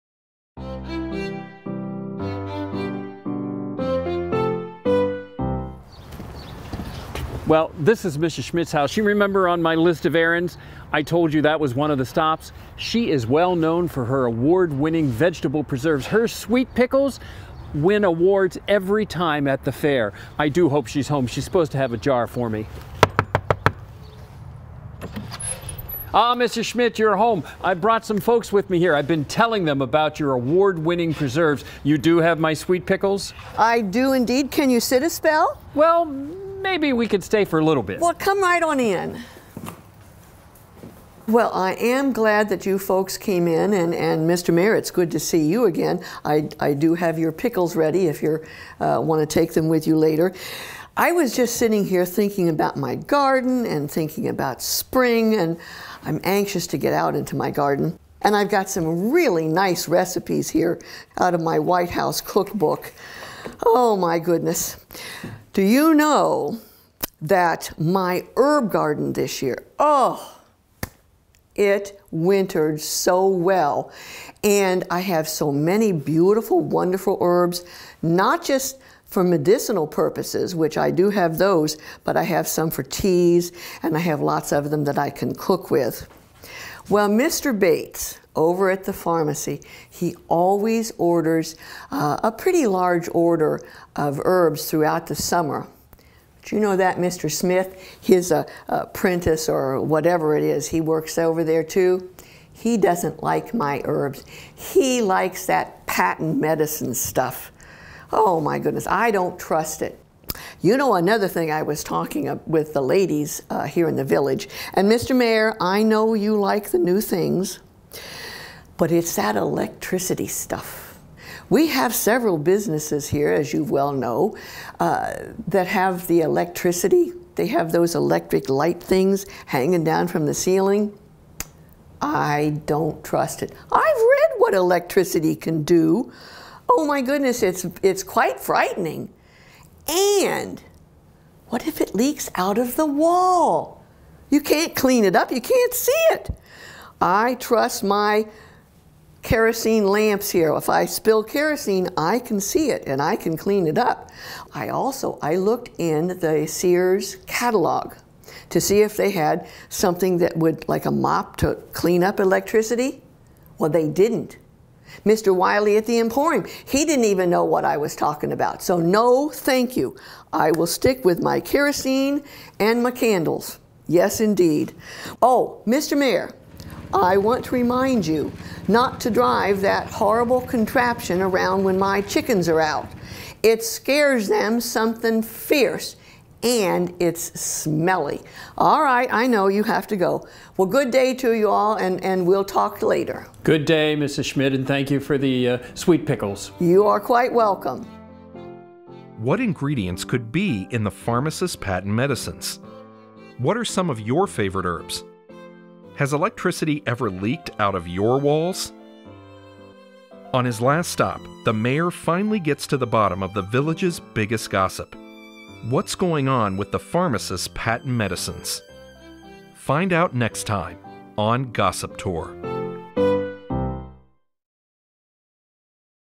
Tales from the Village: Mrs. Schmidt’s Strong Opinions
Mayor O’Fallon stops by Mrs. Schmidt’s house to pick up some of her award-winning sweet pickles, which she serves with a side of sass.